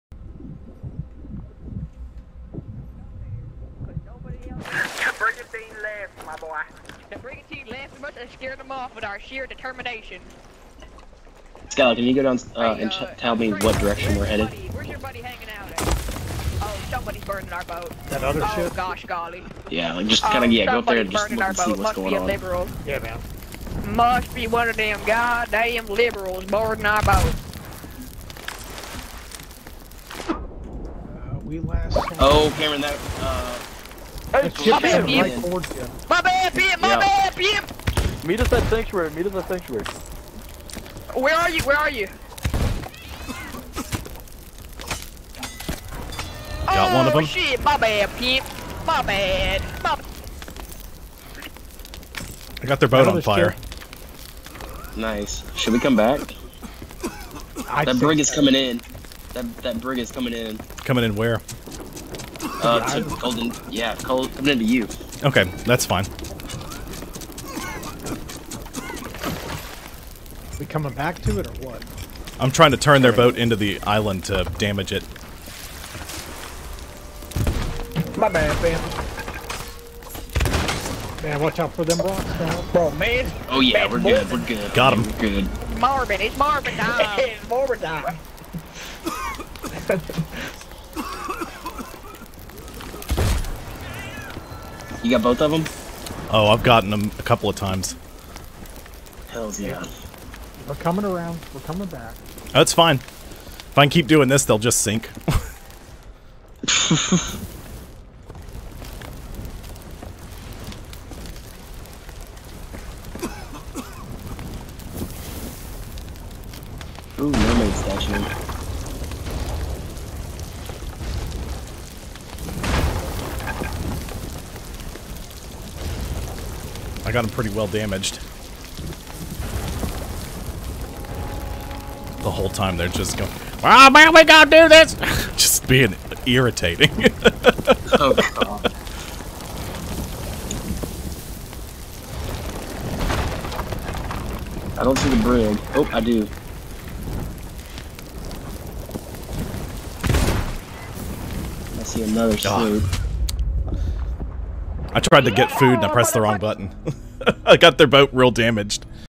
They Are Just Being Irritating
We run across a crew with hot mics just yelling about anything and everything. I almost sink their boat